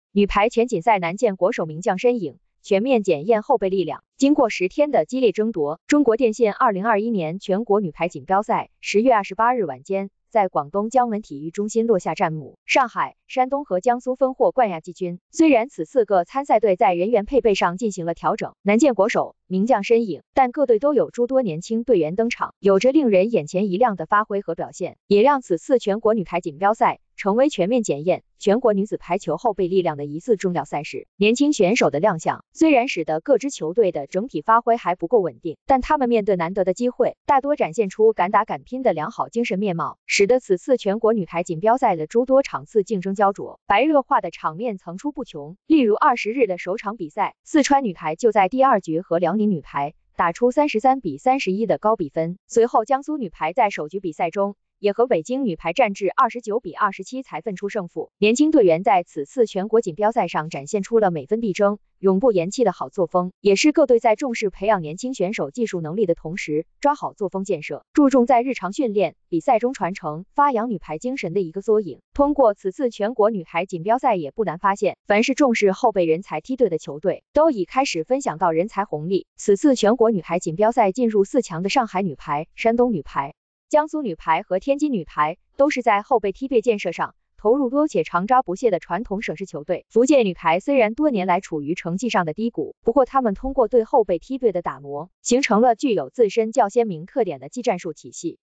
女排全锦赛难见国手名将身影 全面检验后备力量
经过10天的激烈争夺，“中国电信”2021年全国女排锦标赛10月28日晚间在广东江门体育中心落下战幕，上海、山东和江苏分获冠亚季军。虽然此次各参赛队在人员配备上进行了调整，难见国手、名将身影，但各队都有诸多年轻队员登场，有着令人眼前一亮的发挥和表现，也让此次全国女排锦标赛成为全面检验全国女子排球后备力量的一次重要赛事。作为十四运会后的一项全国女子排球大型赛事，由于距离2021—2022中国女子排球超级联赛时间较近，各队都给了连续征战国家队赛事和十四运会的国手以及主力队员休息调整的机会。恰恰因为各支俱乐部都抱着以此次全国锦标赛为锻炼队伍、考察新人的目的，所以各队都在这个新全运周期的“开年之战”给予了年轻选手大量机会。年轻选手的亮相，虽然使得各支球队的整体发挥还不够稳定，但她们面对难得的机会，大多展现出敢打敢拼的良好精神面貌，使得此次全国女排锦标赛的诸多场次竞争胶着，白热化的场面层出不穷。例如20日的首场比赛，四川女排就在第二局和辽宁女排打出33比31的高比分，随后江苏女排在首局比赛中也和北京女排战至29比27才分出胜负。21日的第二轮比赛中，上海女排在第三局和天津女排联袂打出了35比33的高比分，两队27日半决赛再战，也在第三局以31比29才决出高下。年轻队员在此次全国锦标赛上展现出了每分必争、永不言弃的好作风，也是各队在重视培养年轻选手技术能力的同时抓好作风建设，注重在日常训练、比赛中传承、发扬女排精神的一个缩影。后备梯队建设对于中国排球项目而言非常重要。通过此次全国女排锦标赛也不难发现，凡是重视后备人才梯队的球队，都已开始分享到“人才红利”。此次全国女排锦标赛进入四强的上海女排、山东女排、江苏女排和天津女排都是在后备梯队建设上投入多且常抓不懈的传统省市球队。福建女排虽然多年来处于成绩上的低谷，不过她们通过对后备梯队的打磨，形成了具有自身较鲜明特点的技战术体系。在激烈的较量中，诸如天津女排的陈博雅，江苏女排的倪非凡，辽宁女排的孙小轩、石冰彤、孙海平，上海女排的张钰倩、仲慧、许晓婷，福建女排的郑益昕、林越馨、沈阳，山东女排的杜清清、梅笑寒、蔡雅倩、曹婷婷，广东女排的陈佩妍等成名较早的年轻选手，在本次锦标赛上发挥稳定、状态良好，日益成熟的她们已逐渐成长为各队的骨干力量。令人欣喜的是，各队也有更多的年轻选手乃至“00后”在比赛中掀起了“青春风暴”，如上海女排的刘哲宁，天津女排的刘美君、夏思佳，江苏女排的万梓玥、周页彤，福建女排的梁玮凡，北京女排的范泊宁等都在此次全国锦标赛上崭露头角。能否在接下来的新赛季联赛中延续好的发挥和状态，甚至成为中国女排在巴黎奥运周期新的实力增长点，这些年轻选手的表现值得期待。